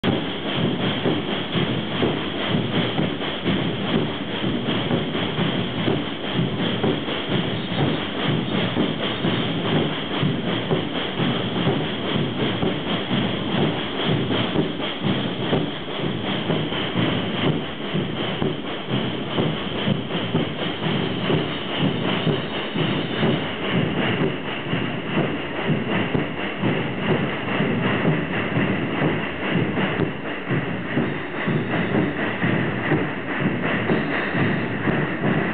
Le Bloco de Samba Résille et la pieuvre
Fête de la musique Toulouse 2009, Samba Résille et sa pieuvre rue Alsace Lorraine